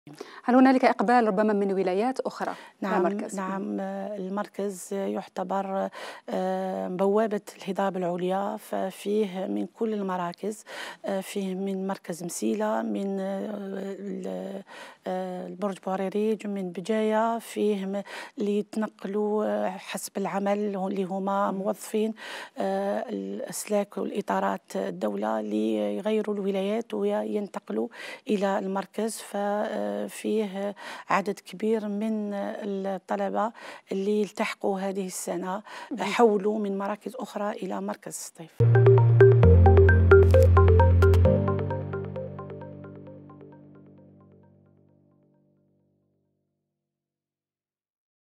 إقبال من ولايات مجاورة على مركز التكوين المتواصل بسطيف
البروفيسور ليلى قطاف
مديرة مركز التكوين المتواصل - سطيف
UFC Setif  جامعة التكوين المتواصل سطيف
يمكنكم مشاهدة برامج جامعة التكوين المتواصل عبر حسابات الجامعة في انستغرام تويتر وفيسبوك.
كذلك يمكنكم متابعة برامج جامعة التكوين المتواصل عبر منصة يوتيوب من خلال الرابط التالي: